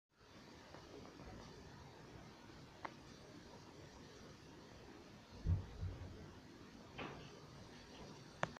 What's your personal opinion on these characters? #22
I love Rudolph the Red-Nosed Reindeer
I like Pupert Pesky, Craig Williams, Chumpkins and Mark (Big City Greens)
I dislike Slenderman
And I hate Millicent Clyde and Mayor Toadstool